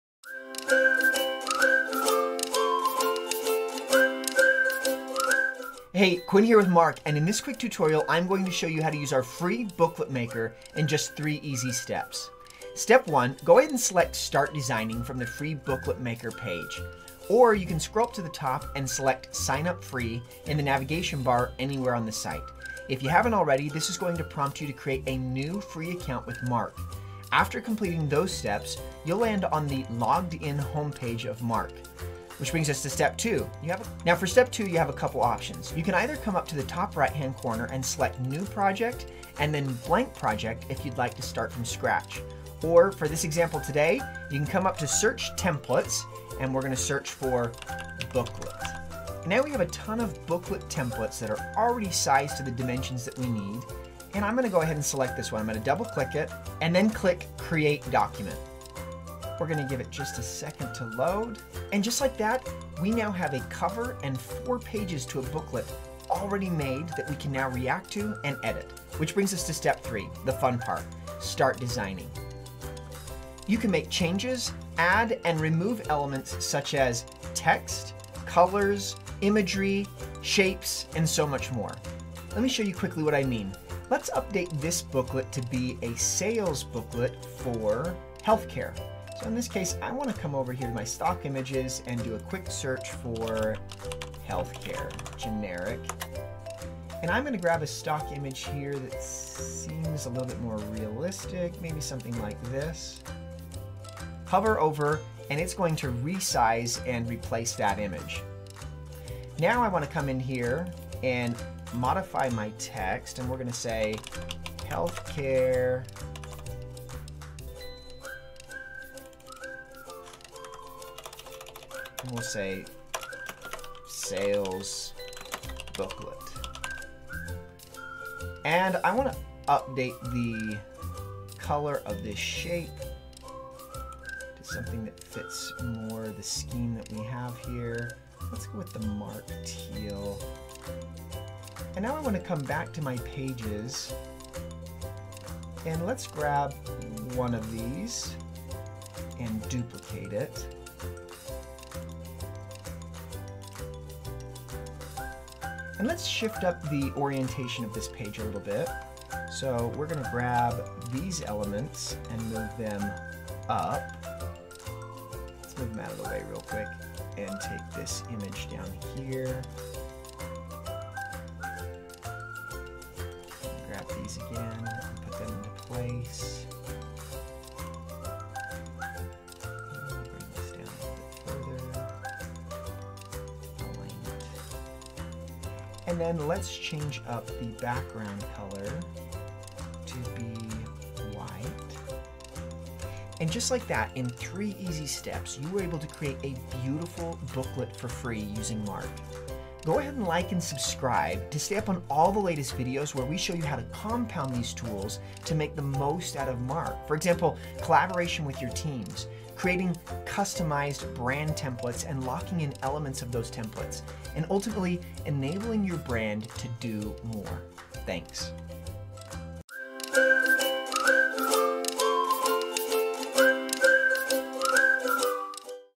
How To Use Free Booklet Maker | Marq
Discover the power of Marq's free booklet maker with our easy 3 step tutorial! In this video, we'll walk you through the simple steps to create professional booklets that engage your audience and convey your message effectively.